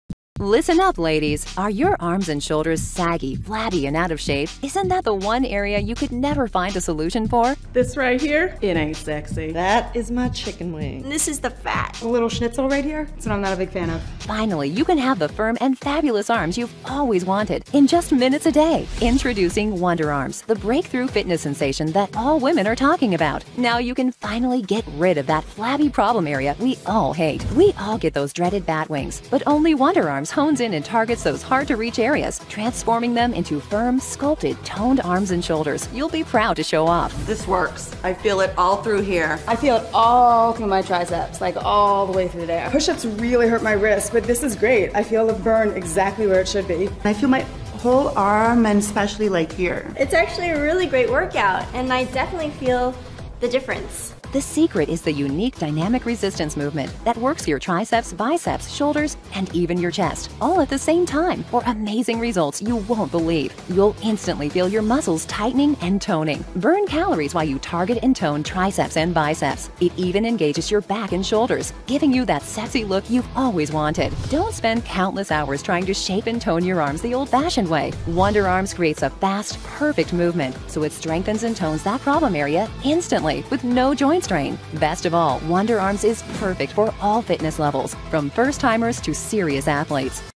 DISPOSITIVO de ENTRENAMIENTO de BRAZOS. 💪
Dispositivo de entrenamiento para brazos. Fortalece tus manos, antebrazos, muñecas, dedos, etc. sin tener que ir al gimnasio.